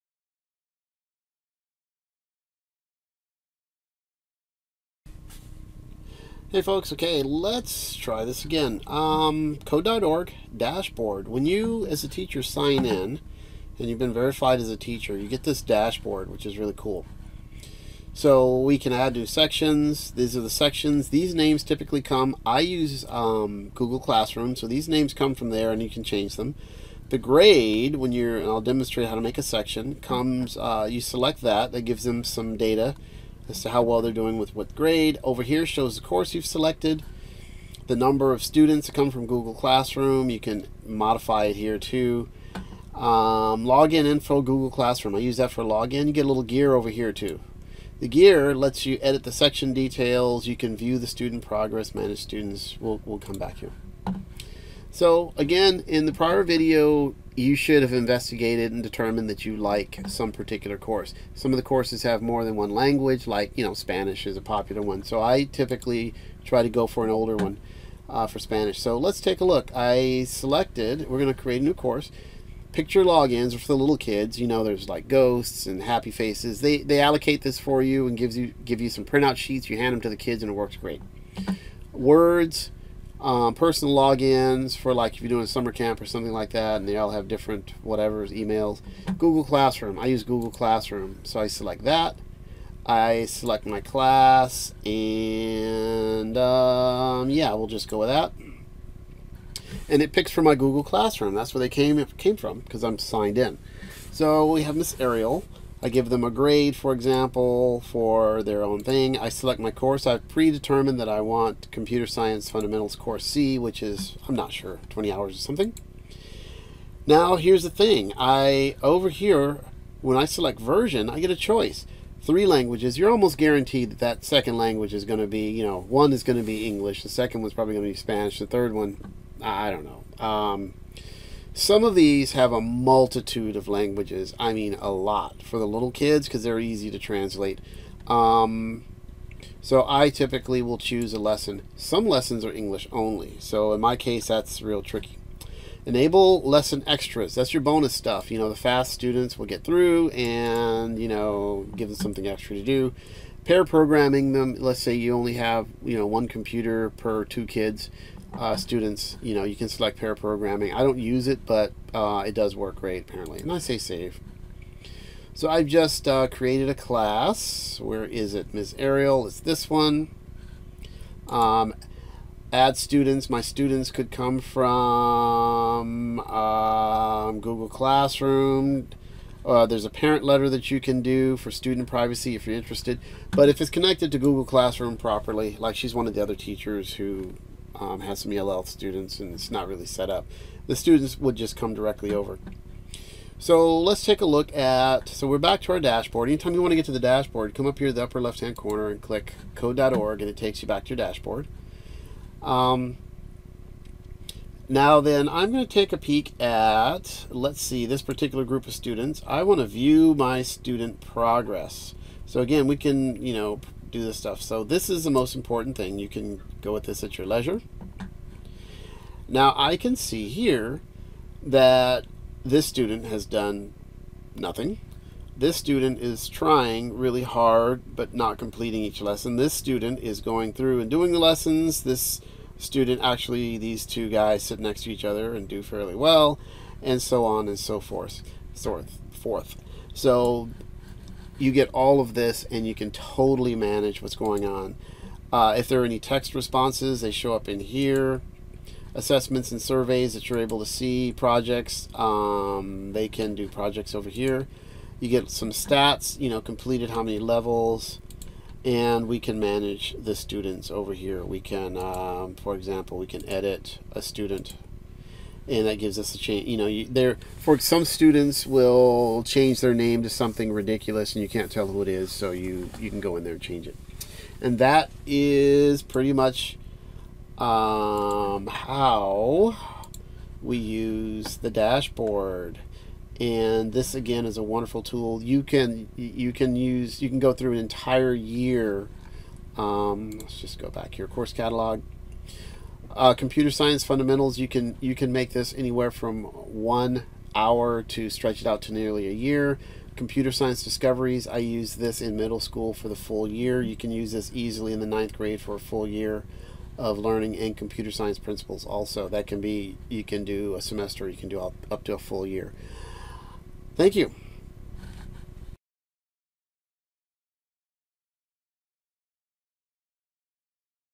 CODE.ORG Teacher Dashboard
This is a quick preview of the teacher dashboard and configuring a section to teach.